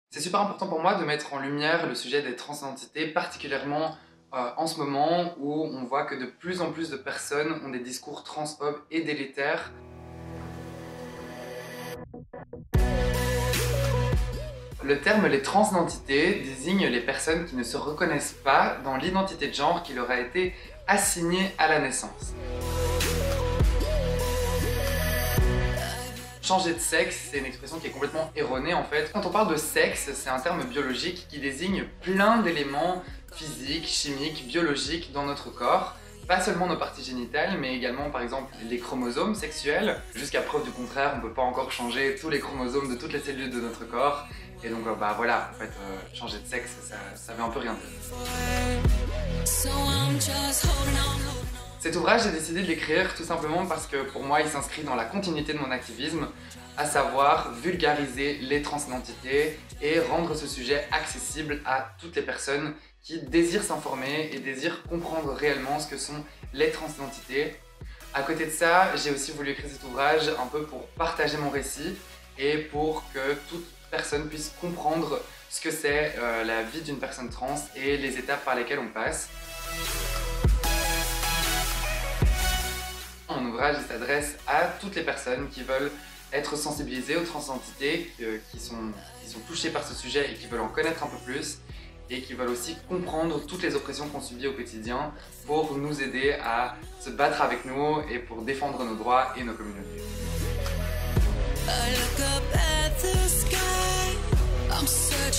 🏳‍⚧ Victoria Defraigne - Les transidentités expliquées à mes parents (et à tous les autres) 📖
👇👇👇 EN SAVOIR PLUS 👇👇👇

🏳‍⚧ Qu’est-ce que la transidentité ? Comment accompagner au mieux un proche dans son coming out transgenre ? Comment éviter les maladresses et se départir des clichés sur le sujet ? Quelles sont les différentes étapes d’une transition ?

🙌 Victoria Defraigne mobilise son expérience et ses qualités de vulgarisation pour expliquer les transidentités à tous ceux qui s’y connaissent peu voire pas du tout sur le sujet. Alternant témoignages et définitions accessibles, elle montre toute la diversité des parcours des personnes transgenres et donne des conseils concrets pour les accompagner, les soutenir et éviter les maladresses. Elle propose également un lexique, qui facilitera la compréhension et l’utilisation d’un vocabulaire pouvant paraître complexe au premier abord.

💡 Un ouvrage pédagogique pour tout comprendre sur les transidentités !